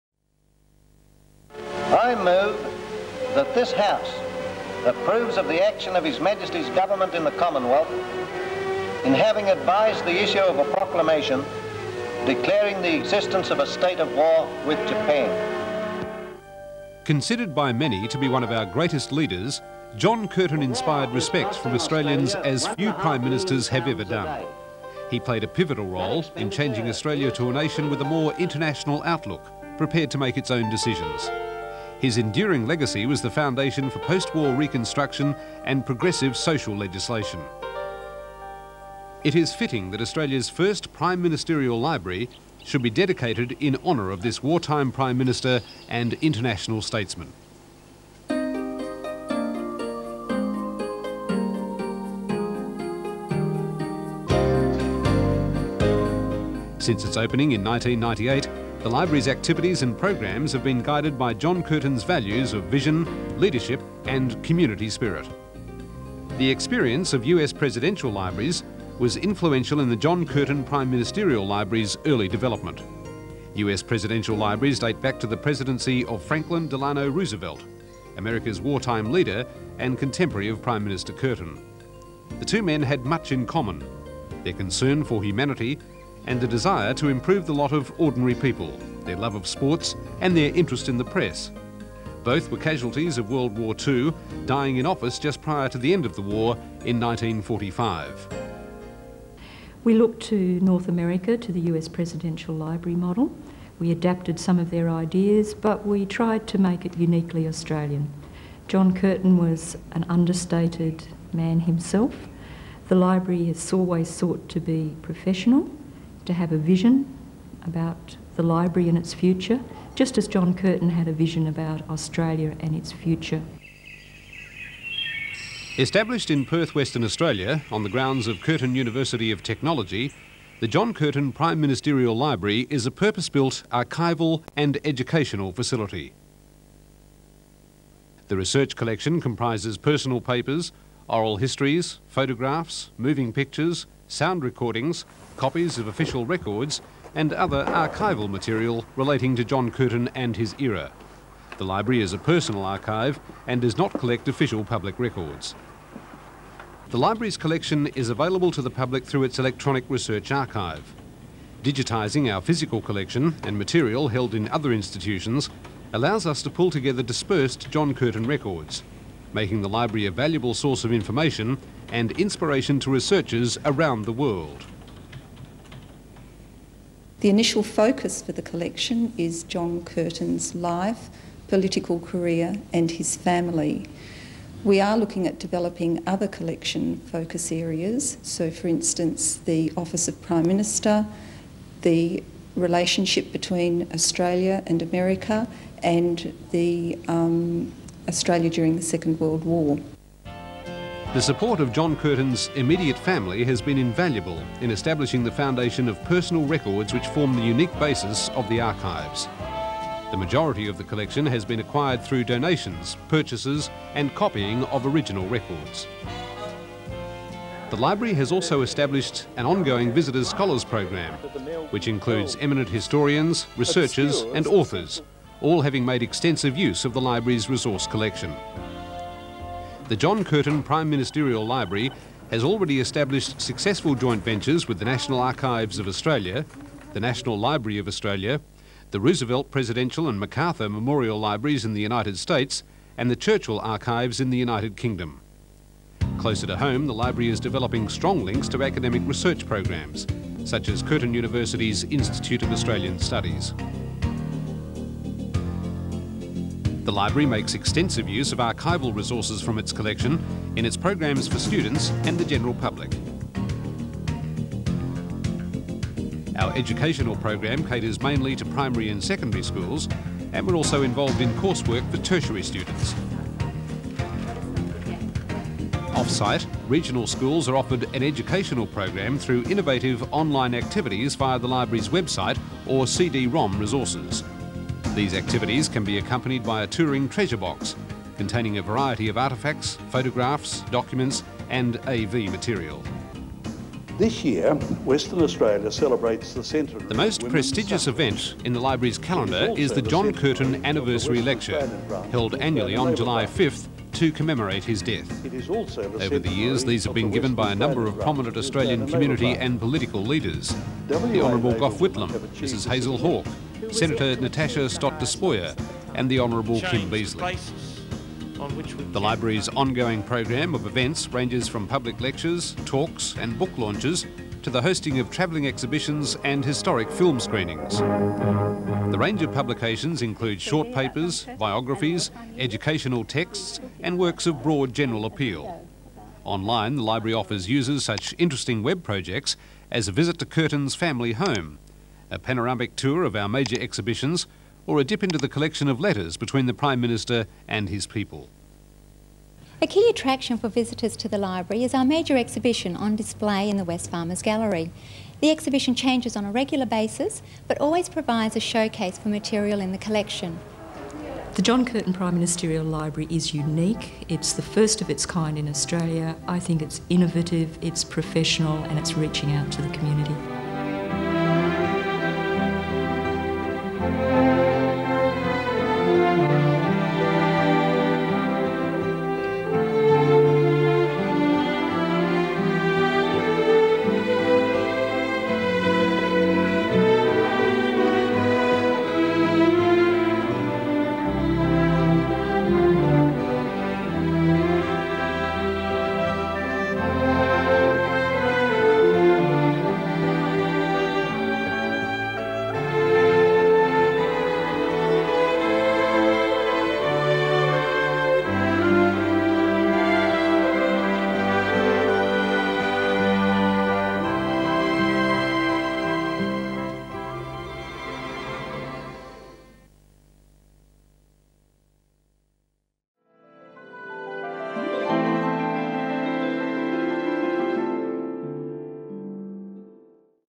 John Curtin Prime Minister Library
Australia's first Prime Ministerial Library dedicated in honour of this wartime Prime Minister and International Statesman.